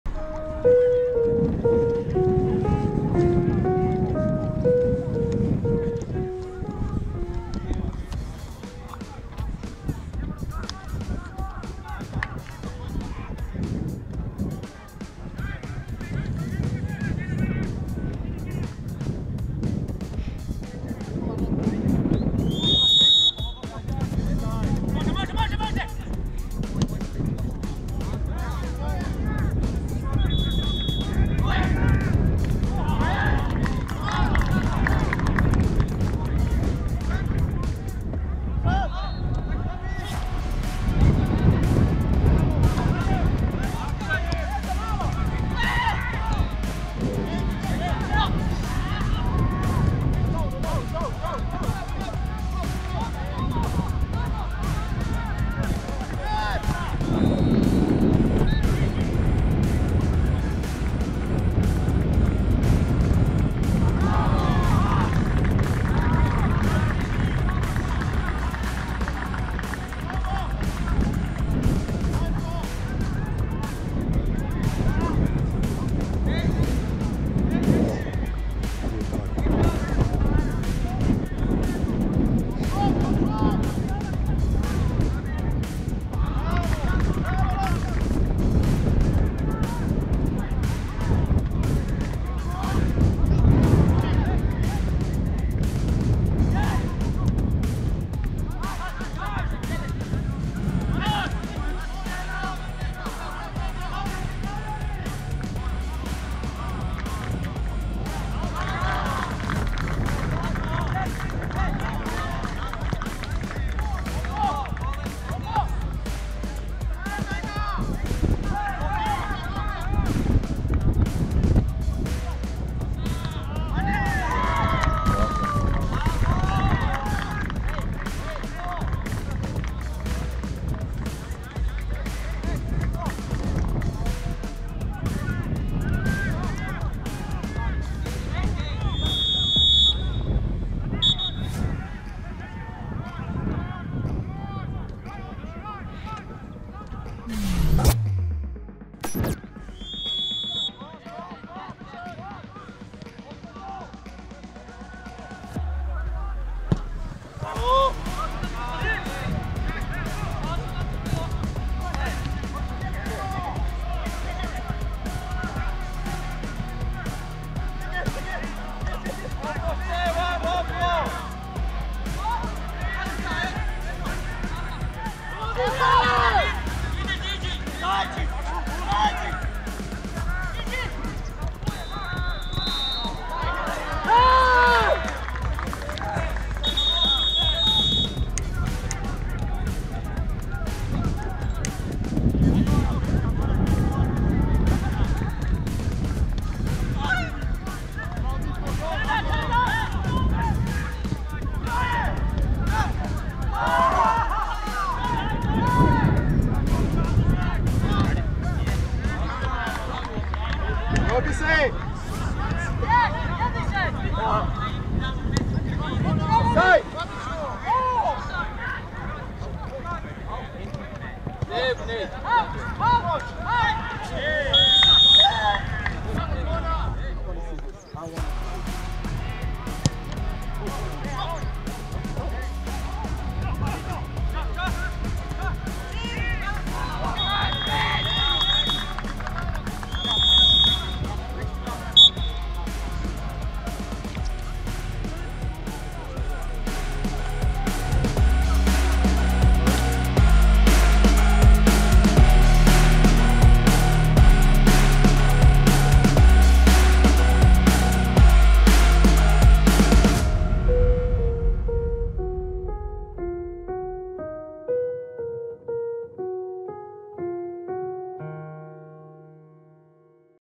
IZVJEŠTAJ I FK Velež - Vejle Boldklub 1:2 I Antalya2024 I 27/01/2024
U dinamičnoj utakmici igranoj na jednom od terena Antalyje Rođeni su poraženi od danskog Vejlea rezultatom 2:1. Jedini gol za naš tim postigao je Nermin Haskić. U priloženom videu pogledajte najzanimljivije detalje današnjeg meča.

FK Velež:
Duka (Nanić) - Kuko (Đuliman), D. Halilović (Zvonić), Mahmić (Oliveira), Domić (Šturm) - Vehabović (Pršeš), Lohan (Mlinarić), Šikalo (Čaić) - Guliashvili (Peco), Suljić (E. Halilović) - Haskić ( Puce)

Besplatno je.